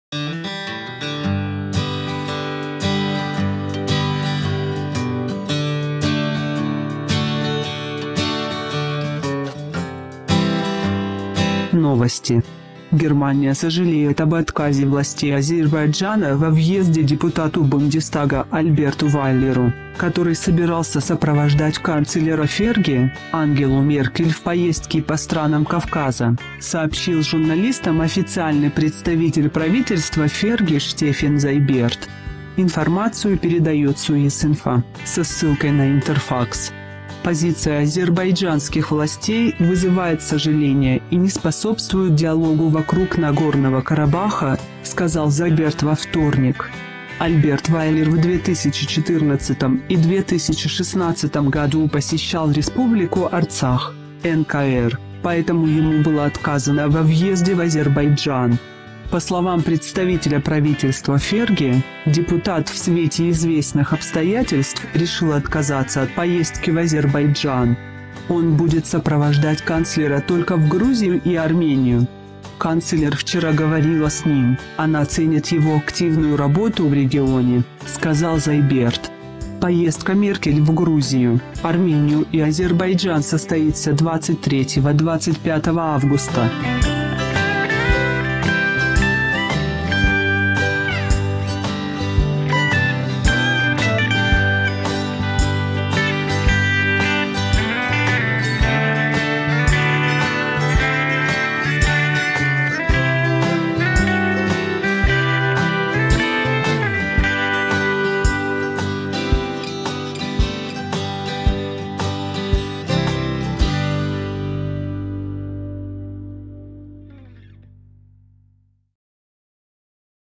В Берлине назвали решение Баку не способствующим диалогу по Карабаху
Берлине назвали решение Баку не способствующим диалогу по Карабаху

В Берлине назвали решение Баку отказать во въезде депутату
Бундестага не способствующим диалогу по Карабаху
Германия сожалеет об отказе властей Азербайджана во въезде депутату Бундестага Альберту Вайлеру, который собирался сопровождать канцлера ФРГ Ангелу Меркель в поездке по странам Кавказа, сообщил журналистам официальный представитель правительства ФРГ Штеффен Зайберт. Информацию передает swissinfo со ссылкой на«Интерфакс».
«Позиция азербайджанских властей вызывает сожаление и не способствует диалогу вокруг Нагорного Карабаха», – сказал Ш.Зайберт во вторник.
А.Вайлер в 2014 и 2016 году посещал Республику Арцах (НКР), поэтому ему было отказано во въезде в Азербайджан.
По словам представителя правительства ФРГ, депутат «в свете известных обстоятельств решил отказаться от поездки в Азербайджан».
«Он будет сопровождать канцлера только в Грузию и Армению. Канцлер вчера говорила с ним, она ценит его активную работу в регионе», – сказал Ш.Зайберт.
Поездка А.Меркель в Грузию, Армению и Азербайджан состоится 23-25 августа.